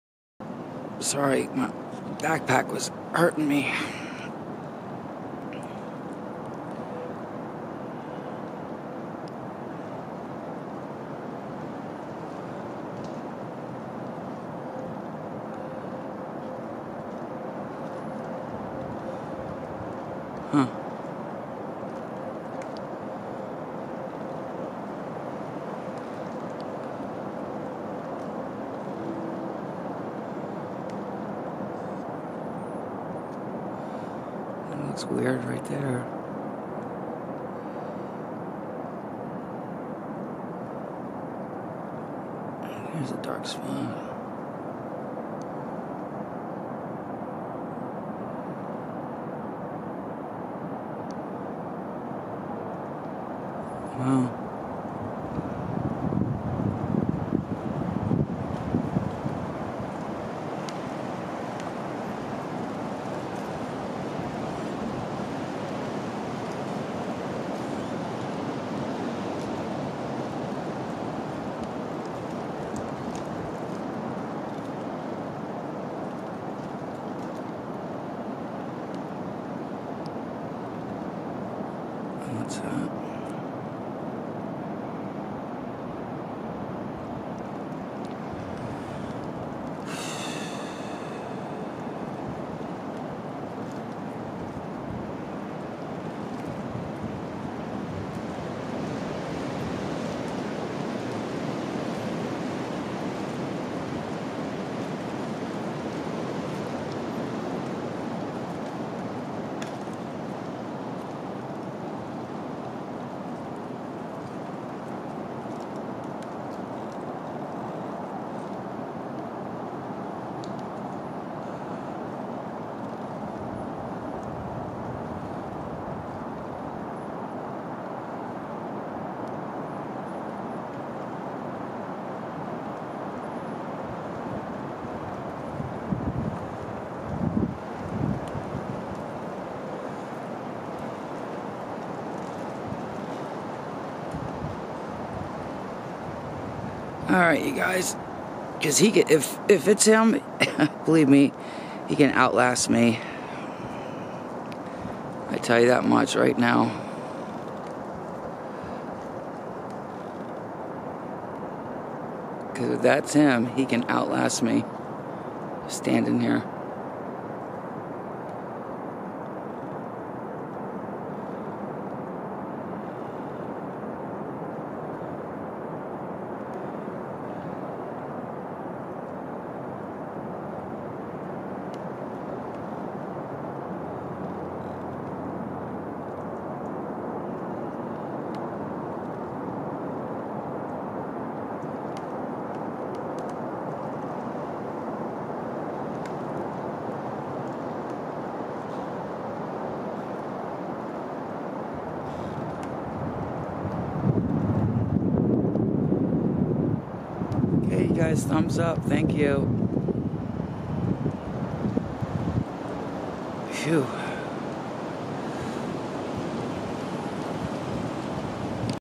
2 BF belly crawling at 1:30,0:05 tree line
Slow & frame to frame..Pay attention to everything in video,in all my videos..